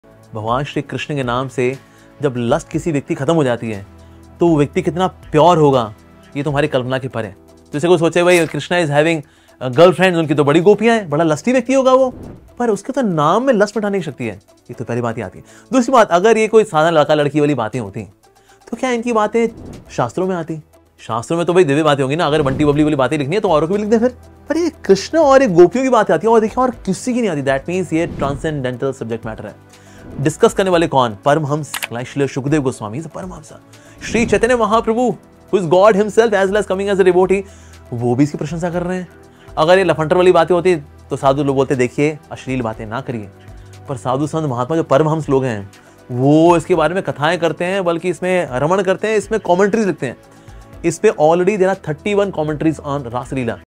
& Krishna | How to understand these all?
Lust , Raslila & Krishna | How to understand these all?
how to control lustcontrol lust subliminal
control lustful thoughts
control lust by iskcon

My Ashraya
ISKCON Delhi
ISKCON